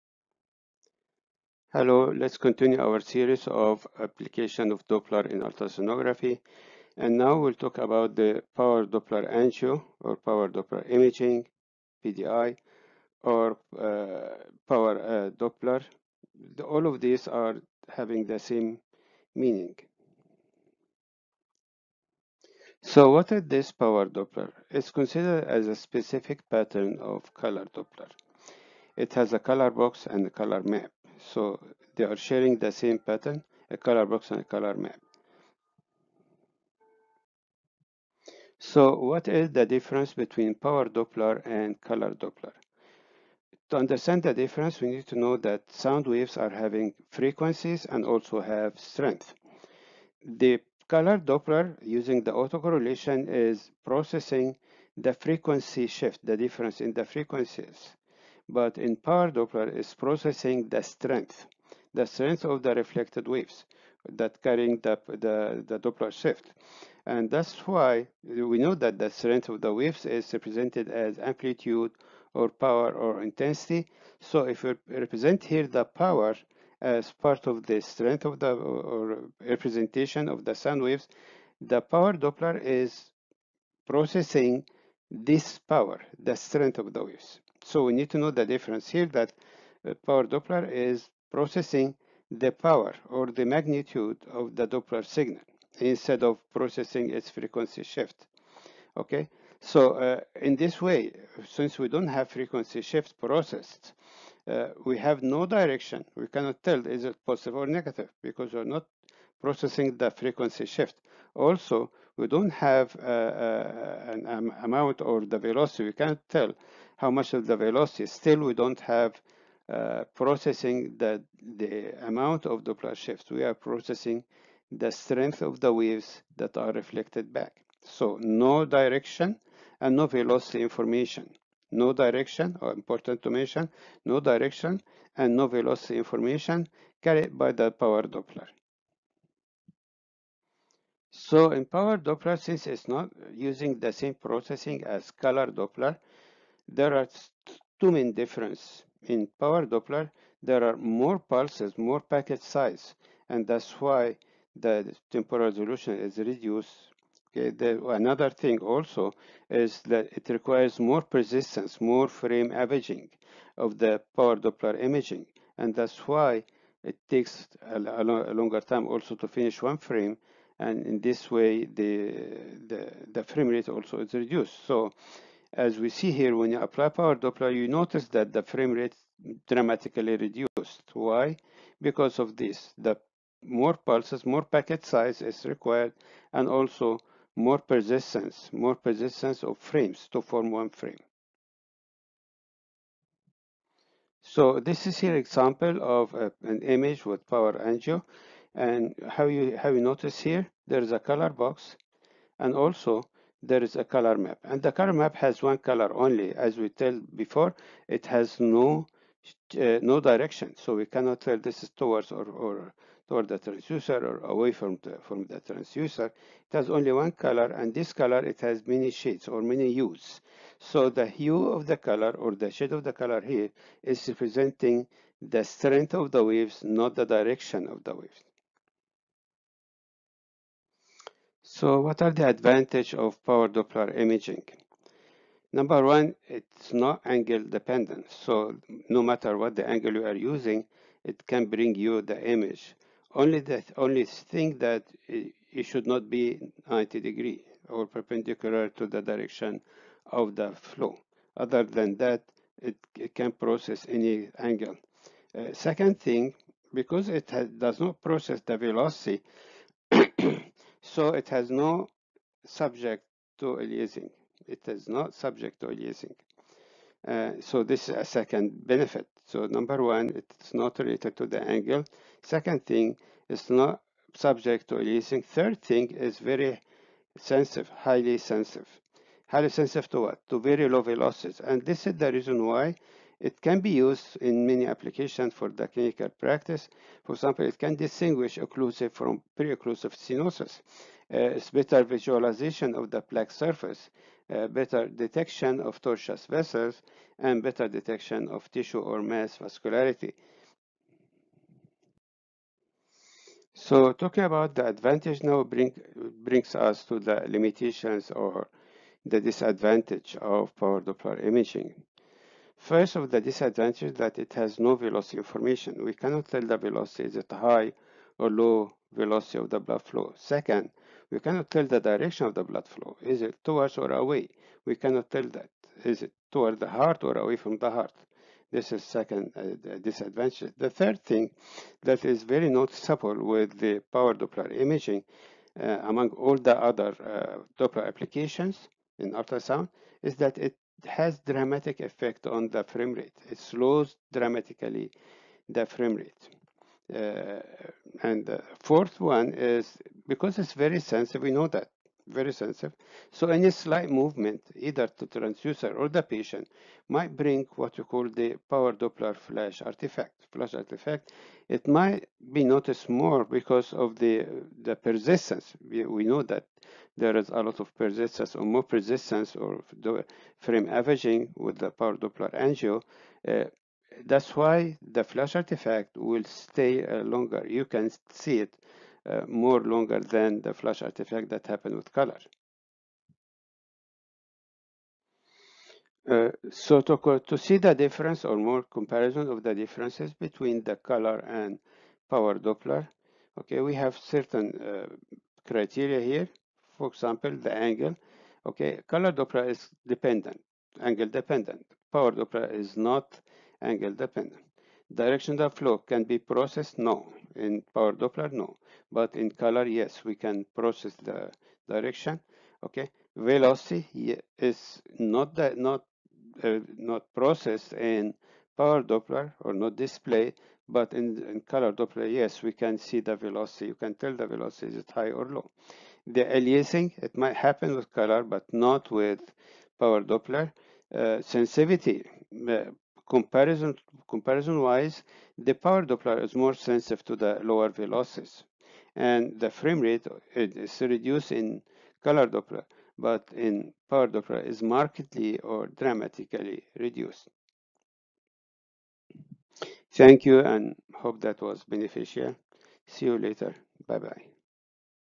POWER DOPPLER
Power Doppler sonography, principles, processing and display